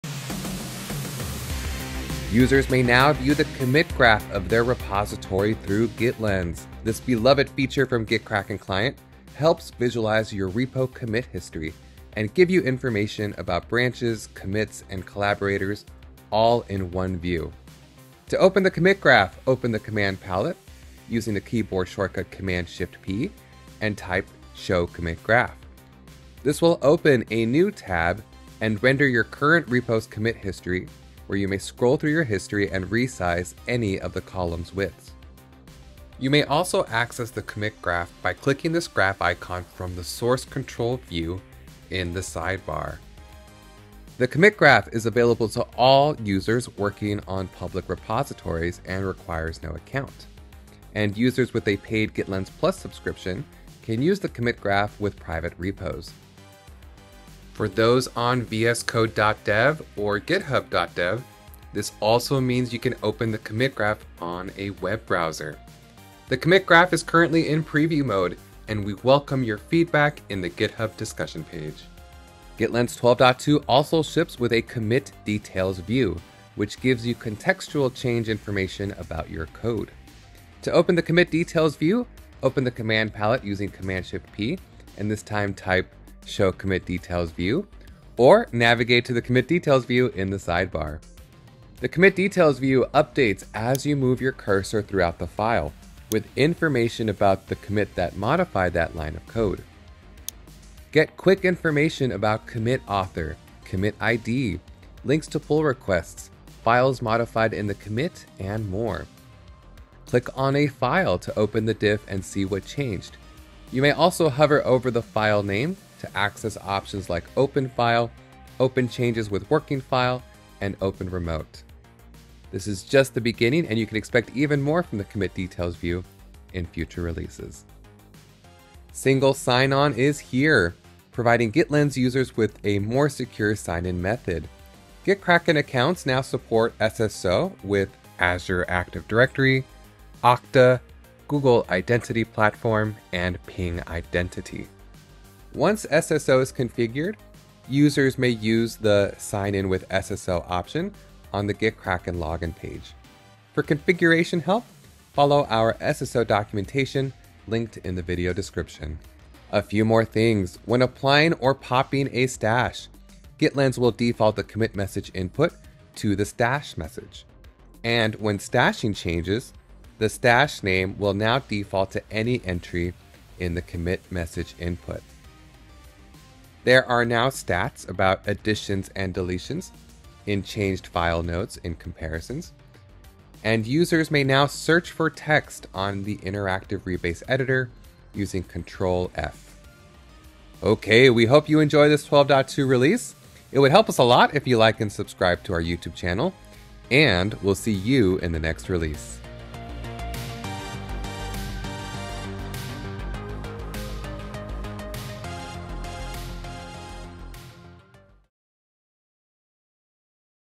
GitLens 12.2 Release: Commit Graph, Commit Details View, SSO & more
Behold! GitLens 12.2 is out with a splash of new features.

Users may now view the Commit Graph of their repository through GitLens+. This beloved feature from GitKraken Client helps visualize your repo commit history and give you information about branches, commits, and collaborators all in one view

To open the Commit Graph in GitLens, open the command palette using the keyboard shortcut Cmd+Shift+P and type “Show Commit Graph.”

GitLens 12.2 also ships with a Commit Details View, which gives you contextual change info about your code. To open the Commit Details View, open the command palette using Cmd+Shift+P and this time type: “Show Commit Details View” or navigate to the Commit Details view in the Sidebar.

GitLens+ users may now integrate with GitHub Enterprise and get avatars, rich autolinks, and links to associated issues and pull requests.

SSO is here, providing GitLens+ users with a more secure sign in method! Your GitKraken account may now initiate an Oauth authentication flow with the following supported Identity Providers (IdPs)

- Azure Active Directory
- Okta
- Google Identity Platform
- Ping Identity

0:00 12.2 release unleashed!
0:06 Commit Graph
0:17 How to open the Commit Graph
0:57 Commit Graph on vscode.dev and github.dev
1:12 Commit Details View in Sidebar
2:08 Single Sign On (SSO)
2:39 Stash name defaults to commit message input
2:56 Other improvements